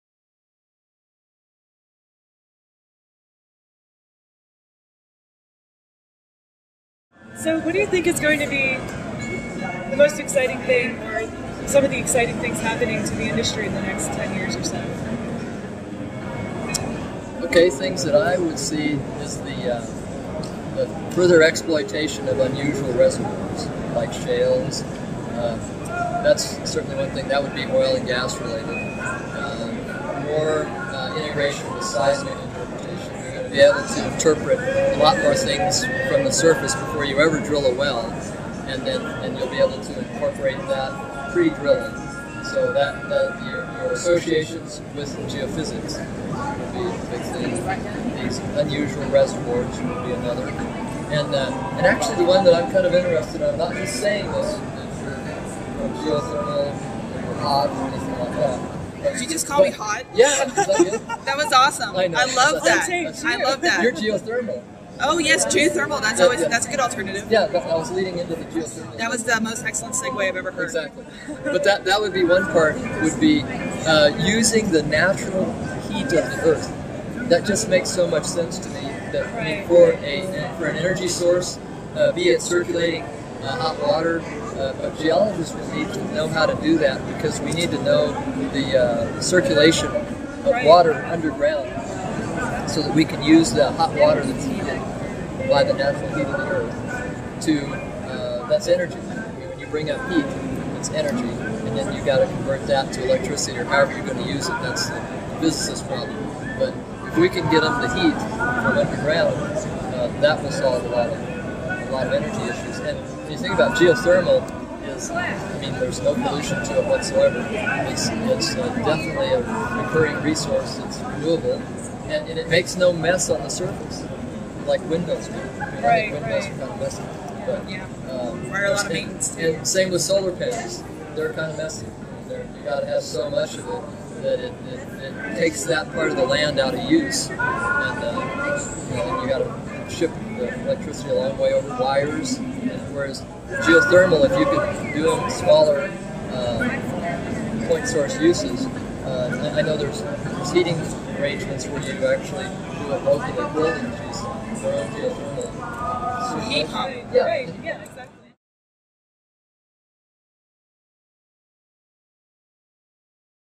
Lee Billingsley Interview, Part 6 of 7
Lee Billingsley talks about exiting things in the oil and gas industry in the next 10 years. Interview with Stephanie Thomas and Meredith Faber, Part 6 of 7, Annual Convention & Exhibition, 2009 Denver.